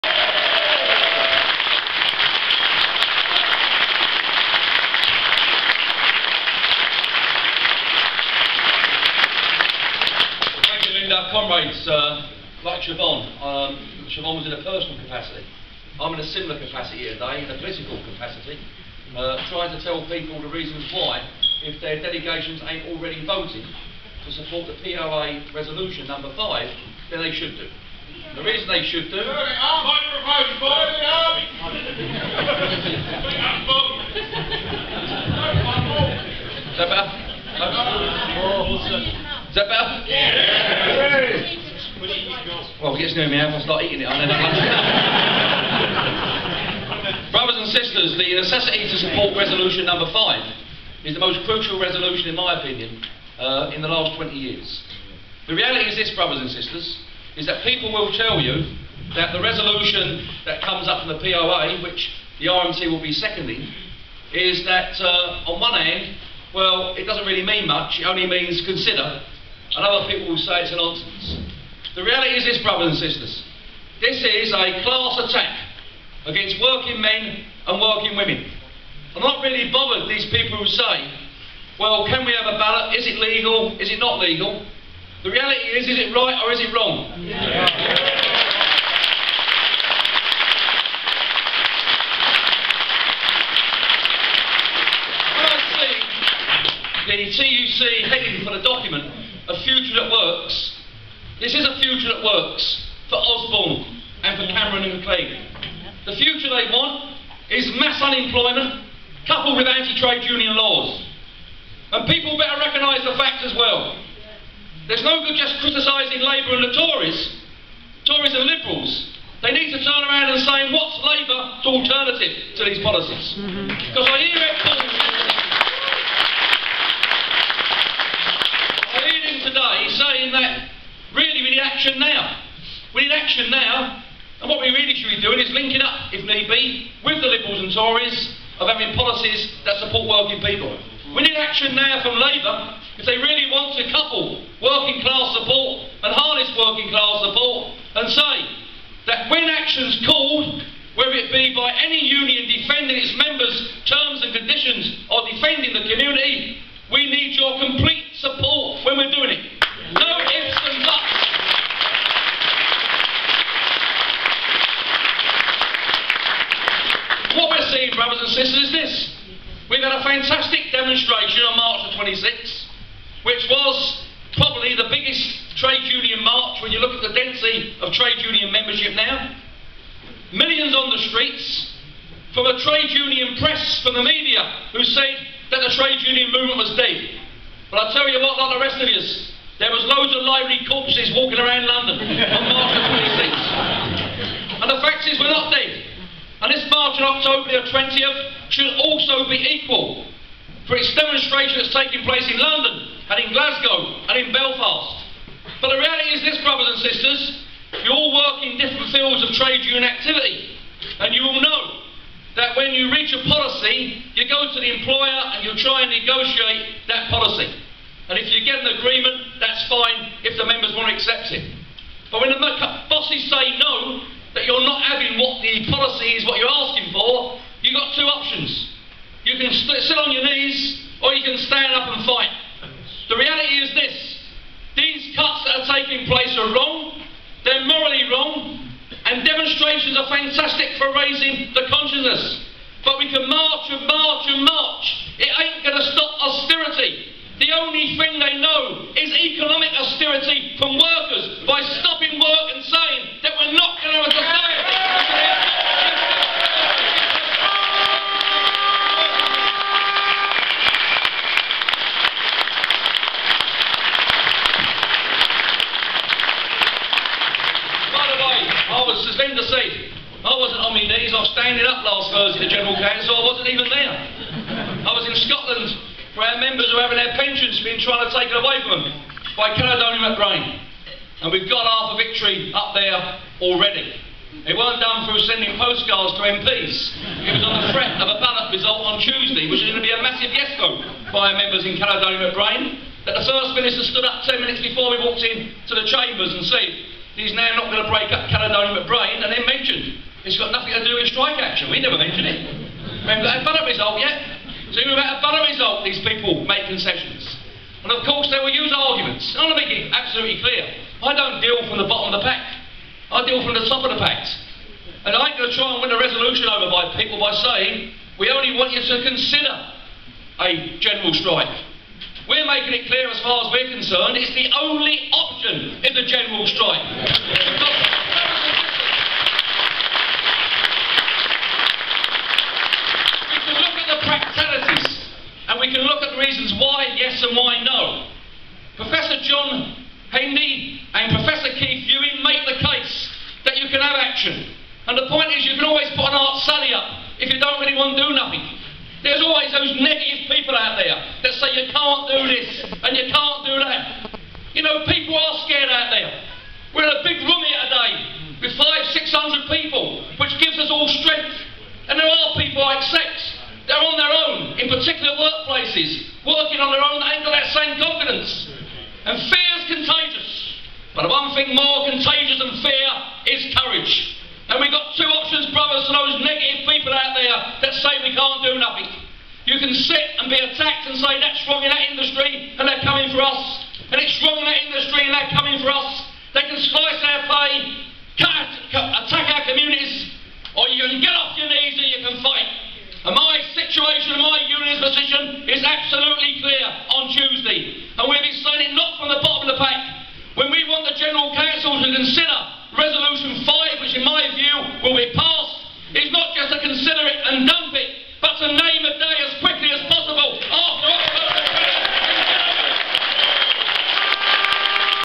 NSSN Lobby TUC Brighton 2012 - Bob Crow, RMT
Second speaker at the NSSN lobby of the TUC in Brighton, calling for a General Strike, was Bob Crow, RMT general secretary. Bob outlined how our class is under attack and the need to fight back with co-ordinated strike action.